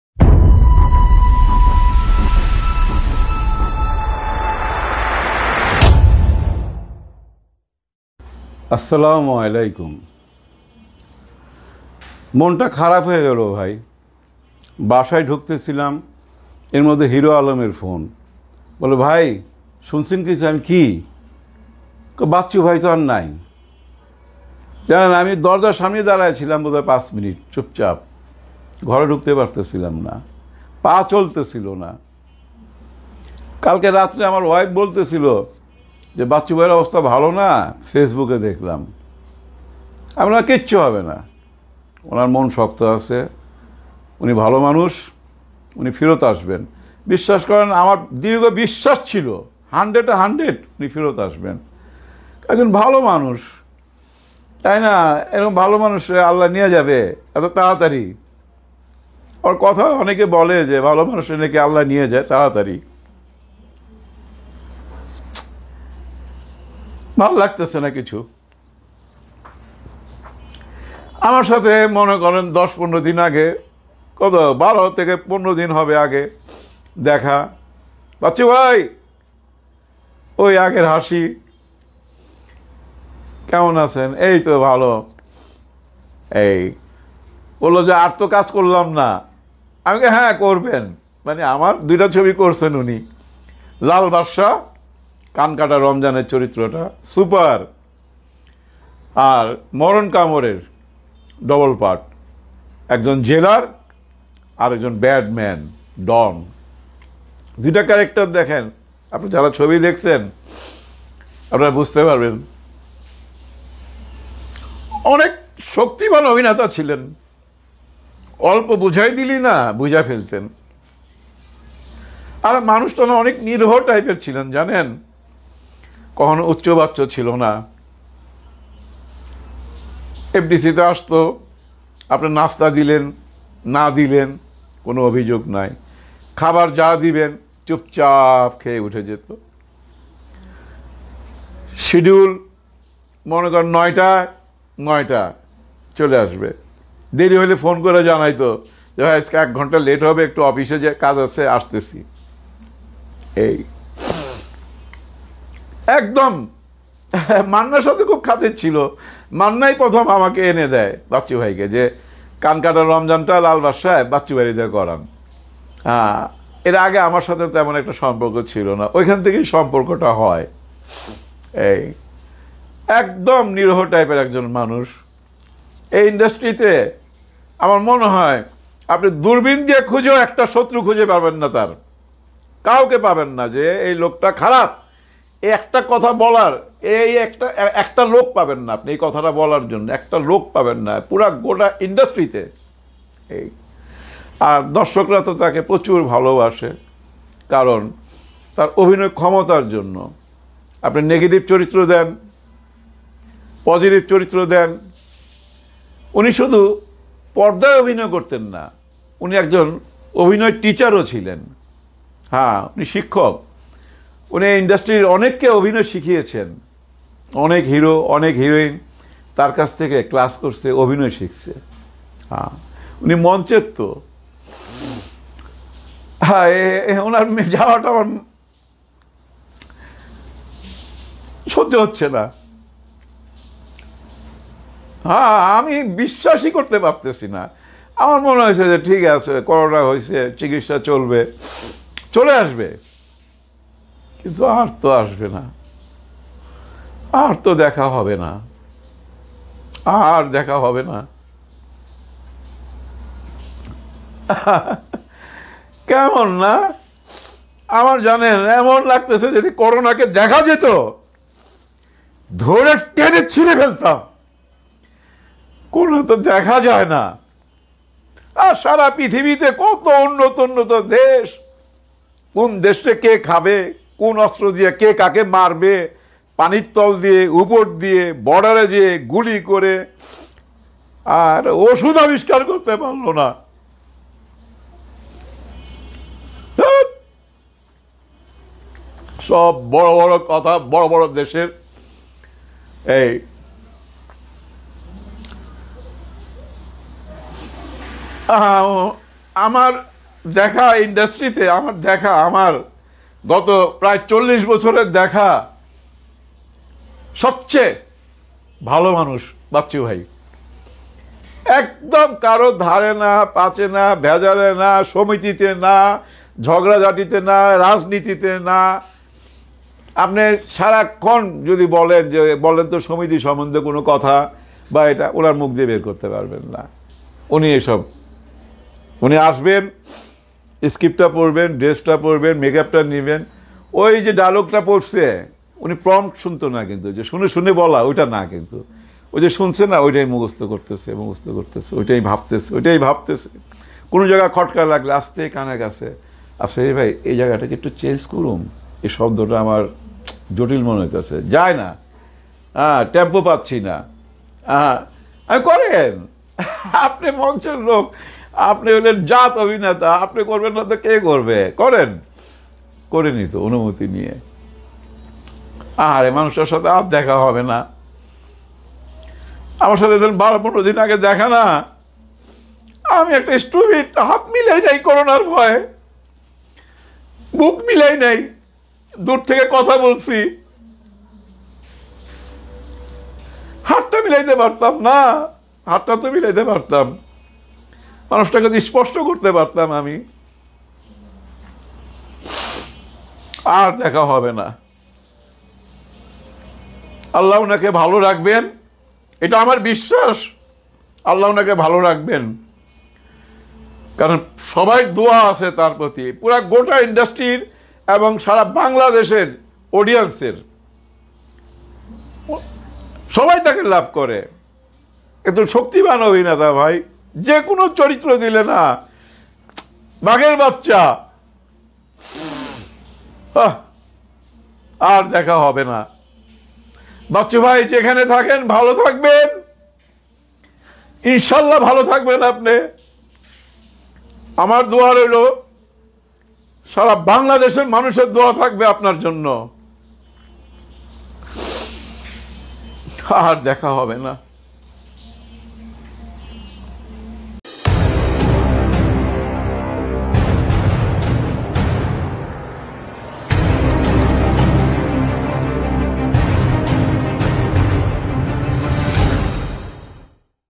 আর দেখা হবে না । সাদেক বাচ্চু l মালেক আফসারী l Sadek Bachchu l Malek Afsary l Bangla New Video 2020
অনেক শক্তিমান অভিনেতা ছিলেন একটু বুঝিয়ে দিলে বুঝে ফেলতেন। মানুষটা অনেক নিরীহ টাইপ এর ছিলেন। কখনও উচ্চ আকাংখা ছিল না। শিডিউল মনে করেন ৯ টা মানে ৯ টা তেই চলে আসবে। এই চলচ্চিত্র জগতে দূরবীন দিয়ে খুঁজলেও তার শত্রু খুঁজে পাবেন না। উনি কিন্তু শুধু পর্দায় অভিনয় করতেন না উনি ছিলেন অভিনয়ের শিক্ষক। এই ফিল্ম ইন্ড্রাস্ট্রির অনেক কে অভিনয় শিখিয়েছেন। ওনার চলে যাওয়াটা আমি বিশ্বাসই করতে করতে পারছি না। বাচ্চু ভাই যেখানেই থাকবেন ভালো থাকবেন আমার দোয়া রইল 😢🙏

অটোমেটিক।

ইউটিউব আইন মেনে চলা খুবই জরুরী।

সবার জন্য ভালোবাসা। 😍